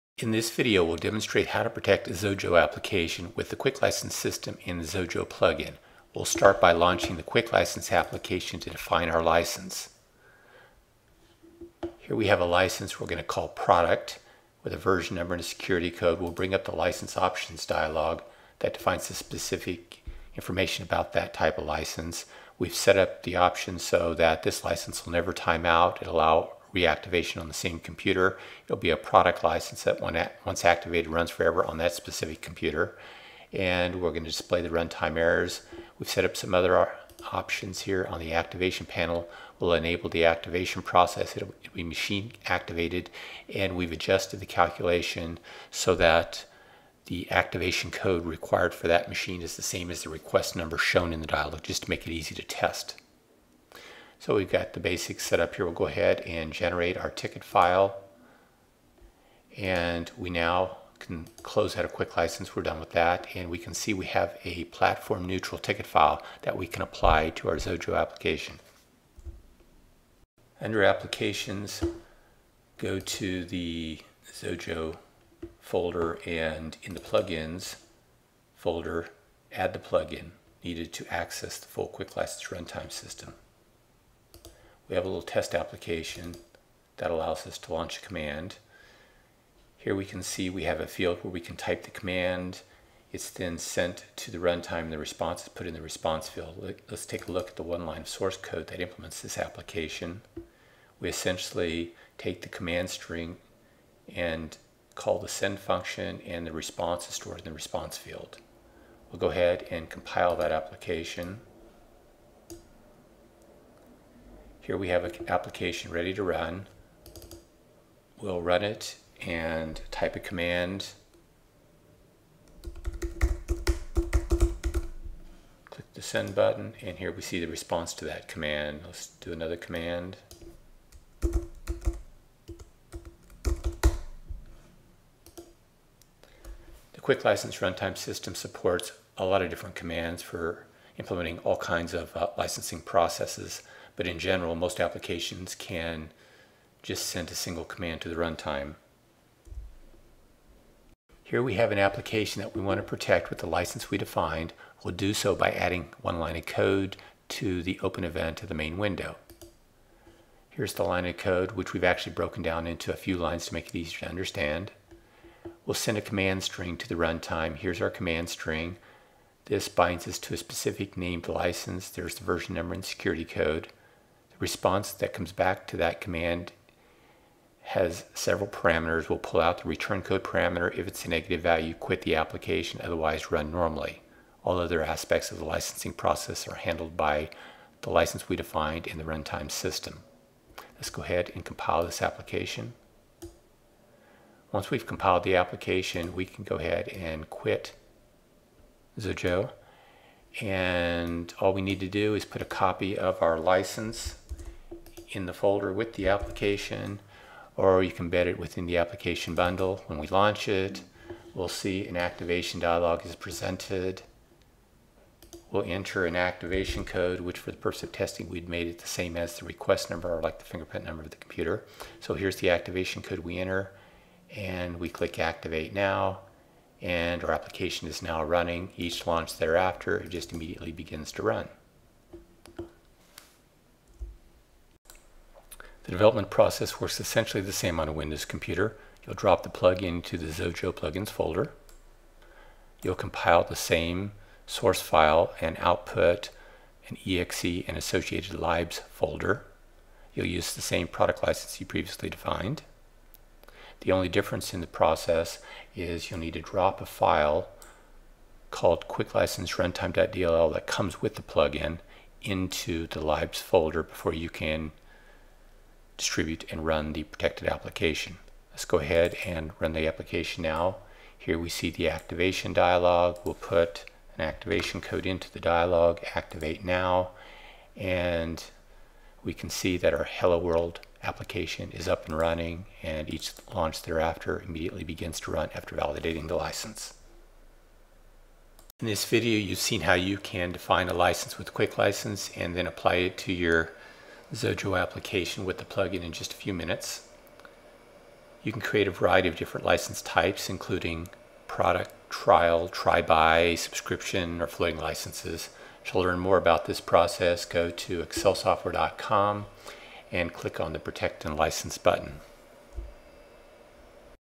Xojo Plugin to Protect and License an Application
Protect an Xojo application with a Product, Trial, Try/Buy, Subscription or Floating license.